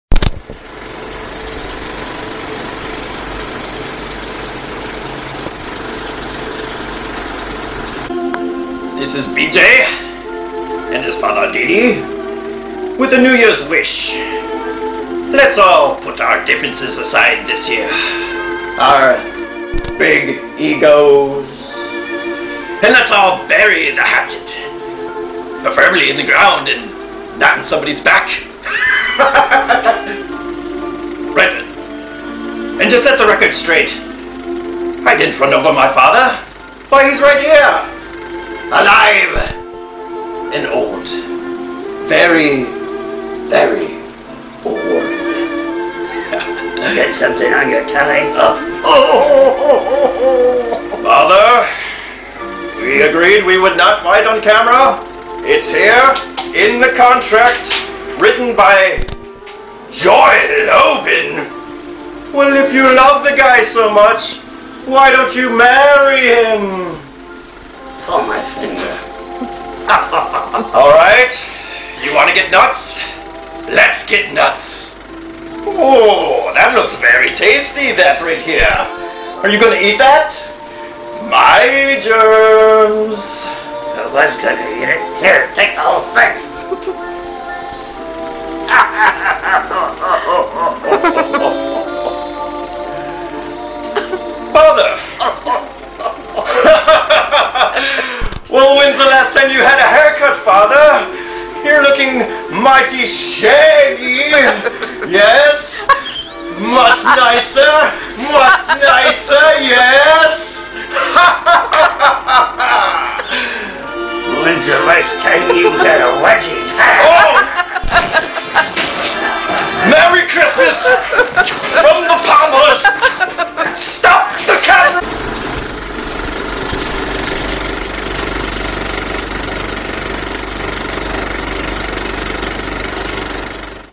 B.J. PALMER & D.D. PALMER BURY THE HATCHET: Chiropractic History Spoof
B.J. Palmer and D.D. Palmer did not get along so well.  They split partnership in 1906 and had a strained relationship.  This video pokes some light fun at an imagined awkward family reunion of BJ & DD.  "Rule number 9, Don't take yourself too damn serious" BJ Palmer.  A little spoof on the Chiropractic History Channel.